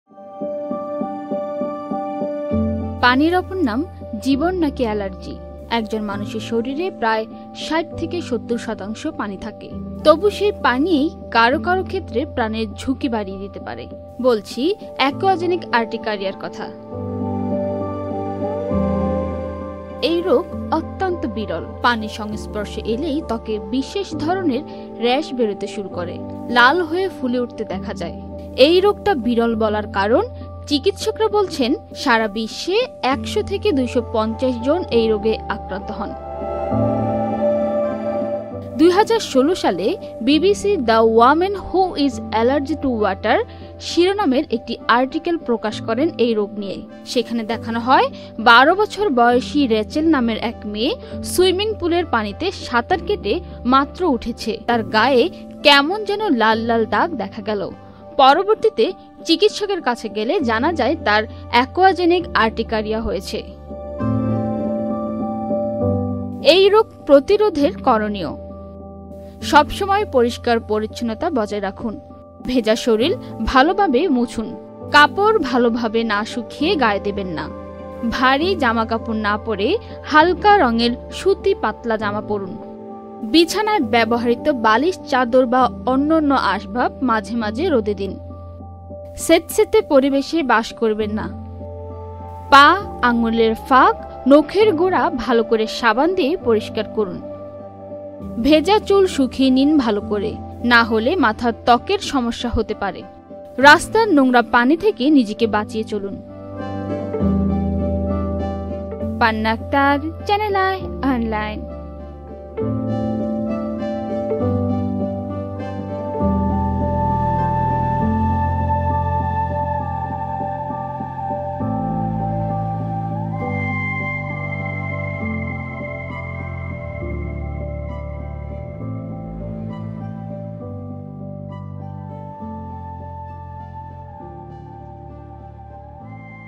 পানিতে নতুন এলার্জি অ্যাকুয়াজেনিক || Channel i News
পানিতে নতুন এলার্জি অ্যাকুয়াজেনিক
এই রোগ অত্যন্ত বিরল। পানির সংস্পর্শে এলেই ত্বকে বিশেষ ধরনের র্যাশ বেরোতে শুরু করে। লাল হয়ে ফুলে উঠতেও দেখা যায়। এই রোগটা বিরল বলার কারণ,  চিকিৎসকেরা বলছেন, খুঁজলে সারা বিশ্বে ১০০ থেকে ২৫০ জন এই রোগে আক্রান্ত হন।

প্রতিবেদক: পান্না আক্তার
সম্পাদনা: রাকিবুল ইসলাম

worldwide.